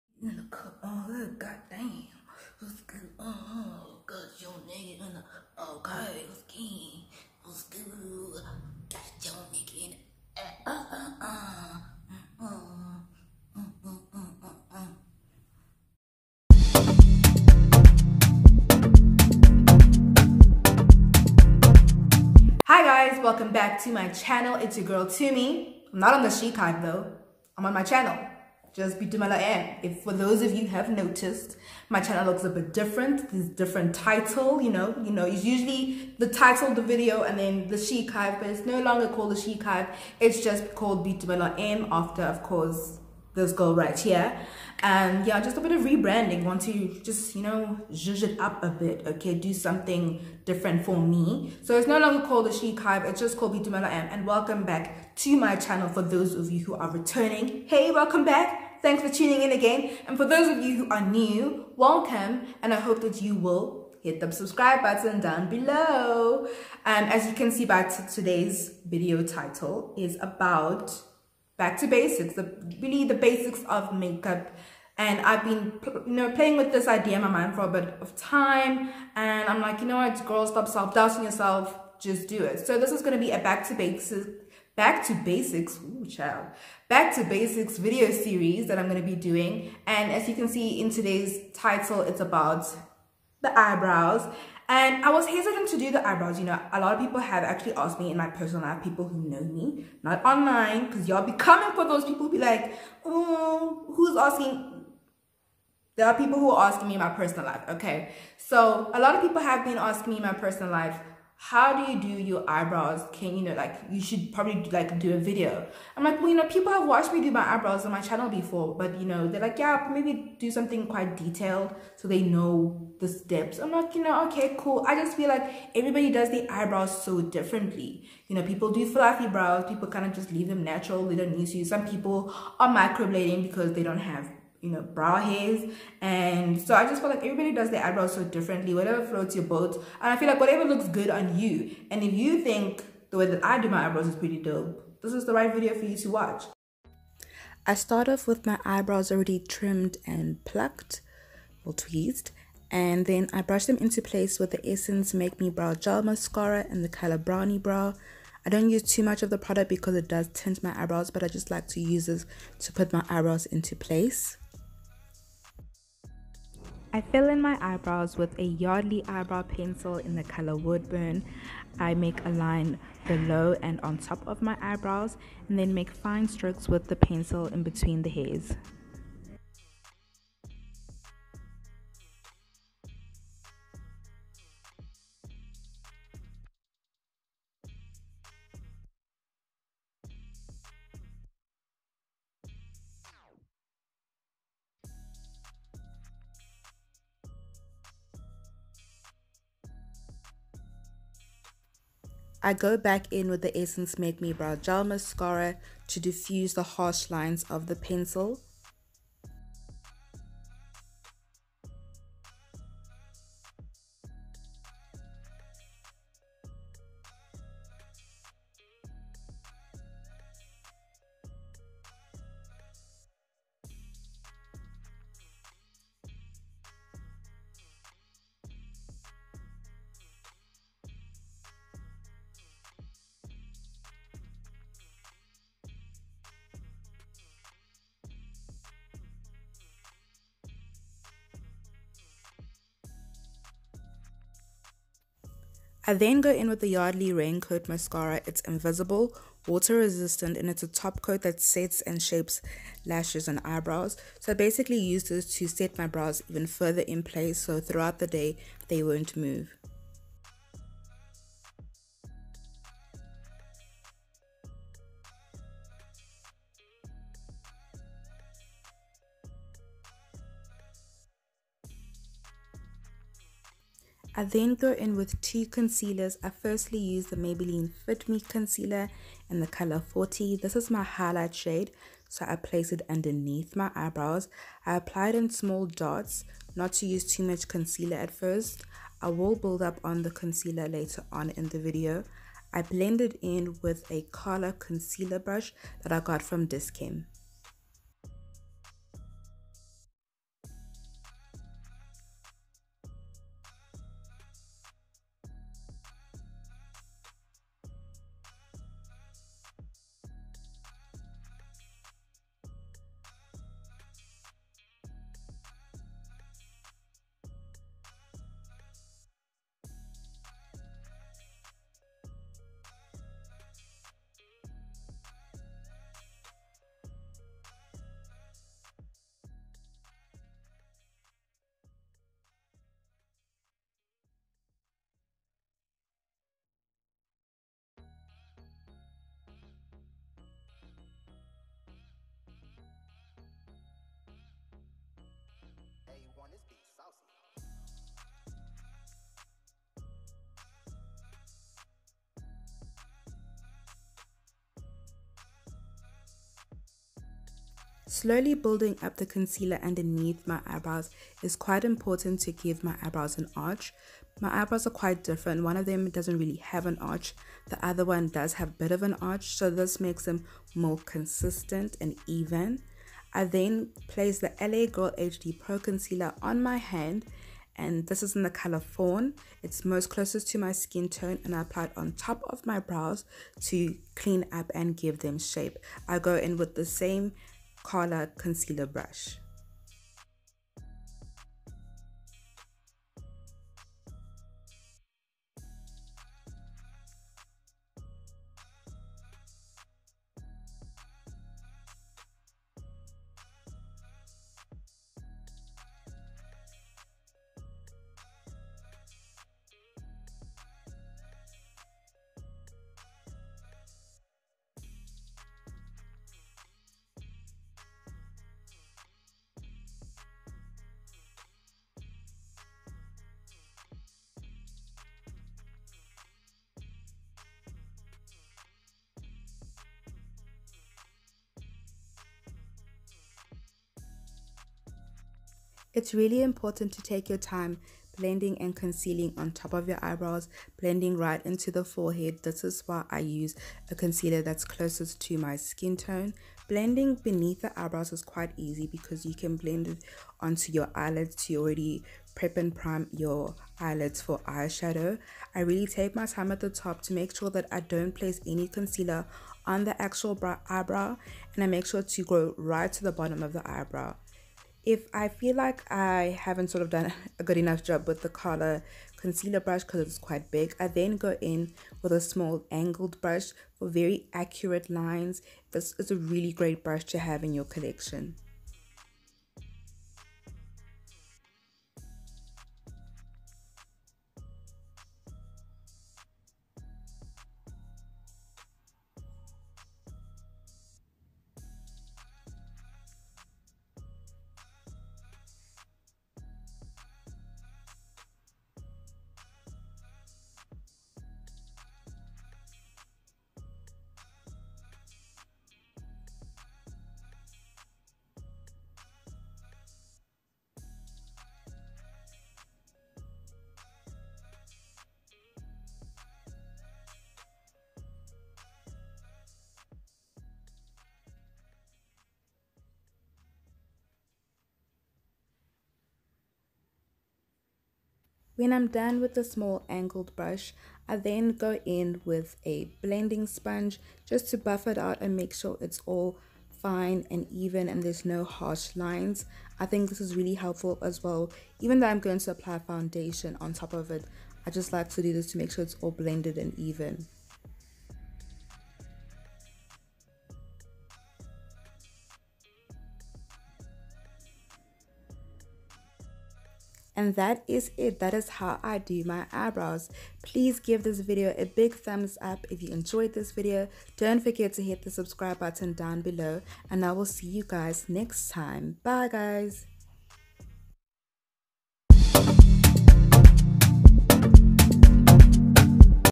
Back to Basics: Eyebrows Tutorial | Boitumelo M | South African YouTuber
Here's the first video of my Back to Basics Makeup series. This is how I do my eyebrows on a daily basis. Simple and easy.

PRODUCTS USED:
Essence Make Me Brow Eyebrow Gel Mascara (02)
Yardley Raincoat Mascara
Maybelline FitMe Concealer (40)
L.A Girl HD Pro Concealer (Fawn)
Cala Concealer Brush
Urban Studio Blending Sponge